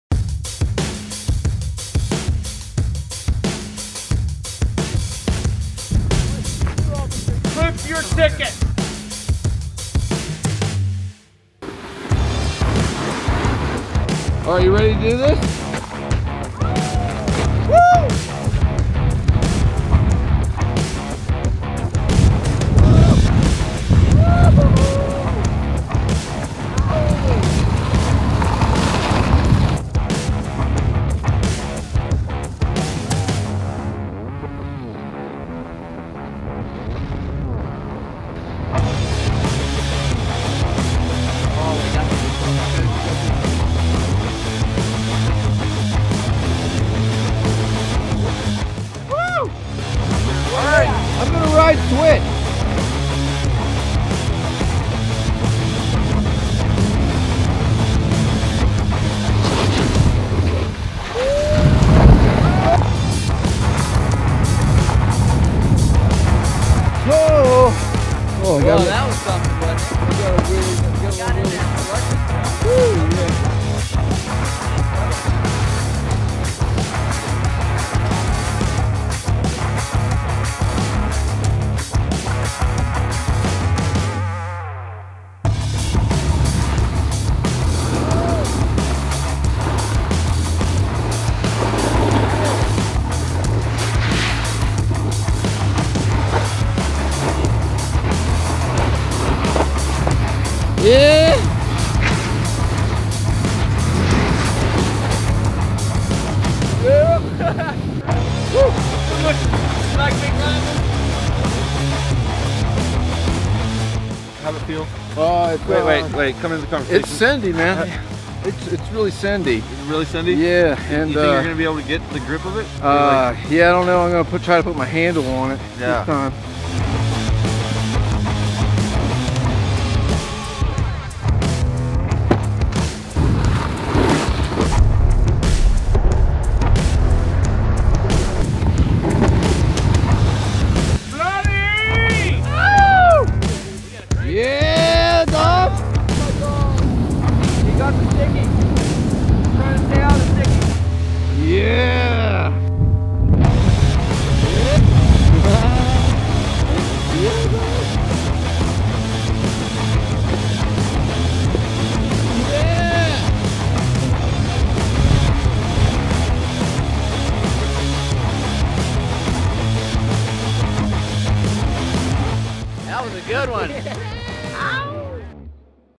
Shreddin' el Wispo. Last time at the Wisp. Sunday, March 8, 2020. 3 weeks ago.
Went shredding with these dudes just before the Wisp closed for the season.. before the world changed. Conditions were epic this year, especially considering that mother nature didn't entirely cooperate. Stay safe everyone. We'll be back next season!